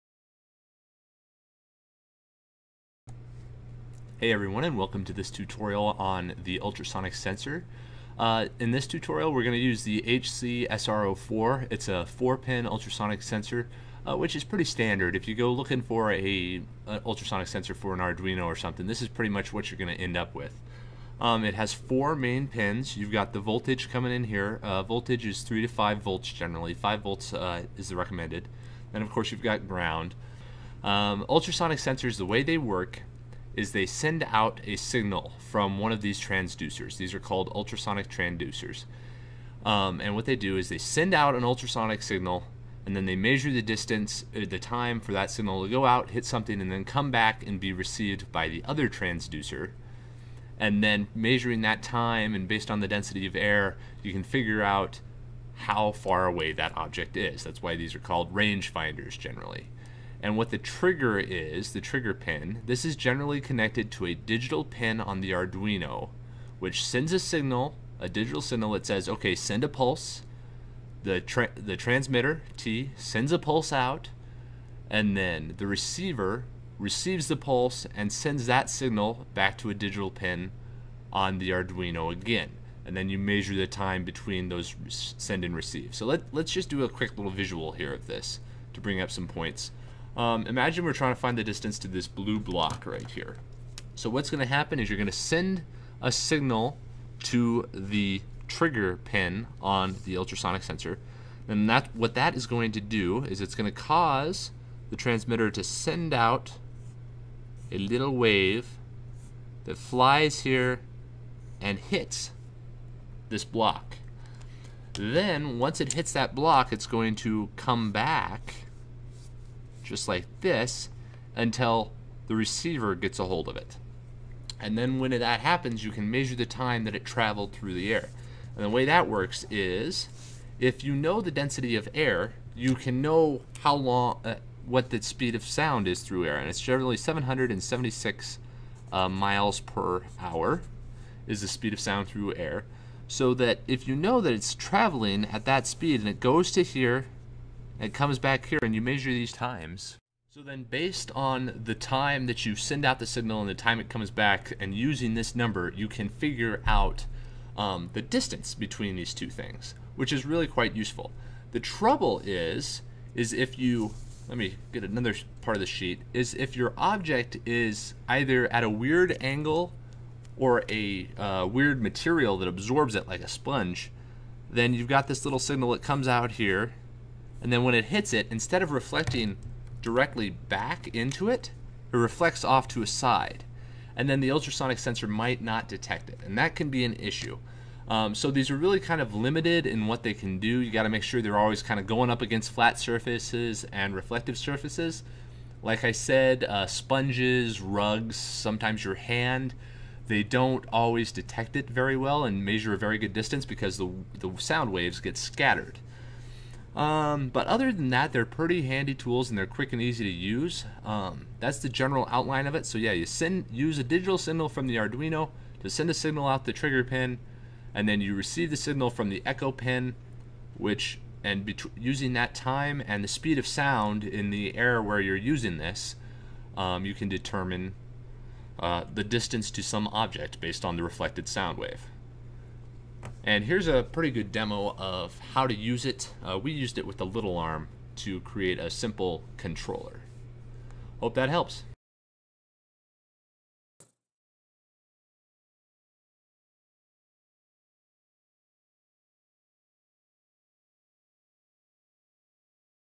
How an Arduino Ultrasonic Sensor Works
In this electronics tutorial we go over how an HC-SR04 ultrasonic distance sensor works with the arduino to determine a distance to an object.

The basic idea is that it send out a sound wave that is reflected by object. Then my measuring the time for the wave to make the trip you can determine distance. We have used the sensors to control the LittleArm arduino robot arm.